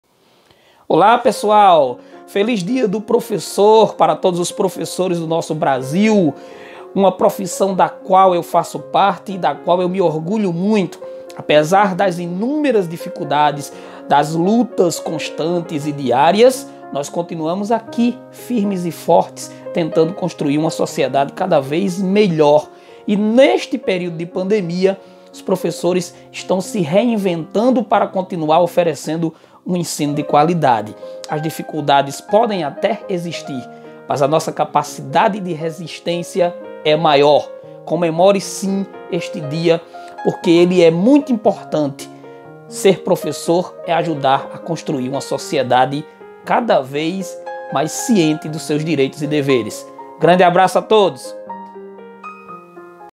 Prof. Cassildo Souza - Homenagem ao DIA DO PROFESSOR 2020
Nossa homenagem ao Dia do Professor, ofício do qual eu me orgulho. Parabéns a todos os professores do Brasil!